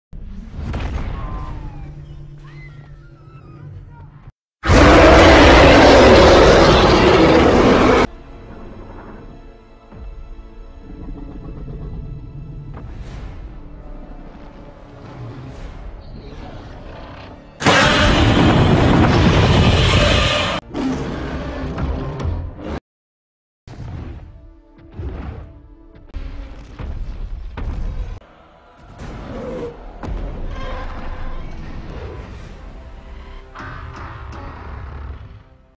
Мини-Трейлер к моему! будущему мультфильму! Годзилла против Конга.
Ребята у меня теперь есть расписание выходов роликов а именно
вторник!:
четверг!:
и может быть суббота или воскресенье
как получится!?